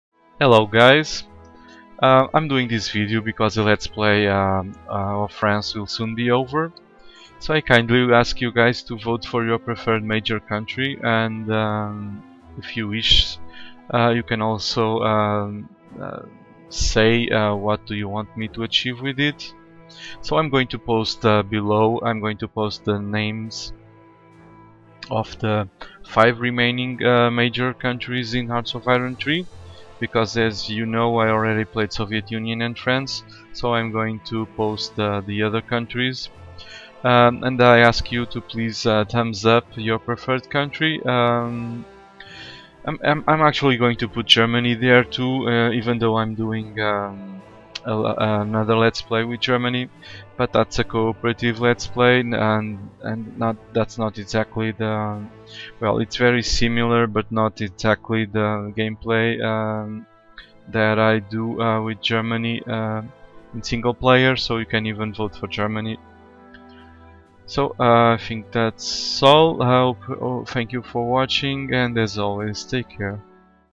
Please chose your major country
The France LP will soon end so I want to know which major country you would like me to play with. Please thumbs up your preferred country; you can also post what you want me to do with it that I will do my best to achieve it. Thanks! :)